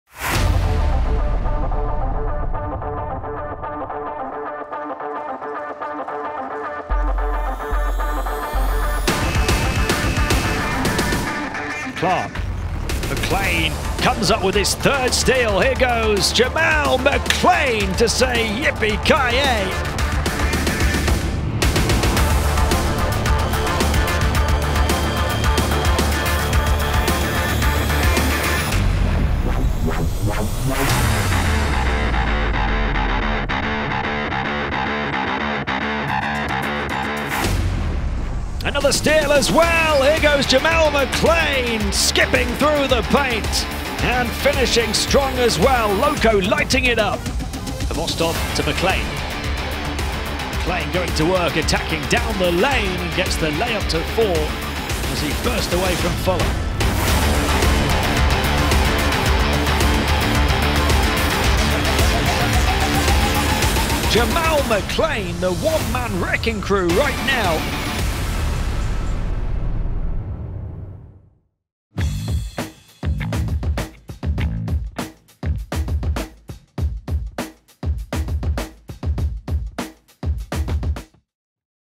7DAYS EuroCup Top 16 Round 2 MVP: Jamel McLean, Lokomotiv Kuban Krasnodar
Lokomotiv Kuban Krasnodar went on to grab its first Top 16 win with a commanding road success against Fraport Skyliners Frankfurt led by Jamel McLean, whose PIR of 31 earned him Round 2 MVP honors.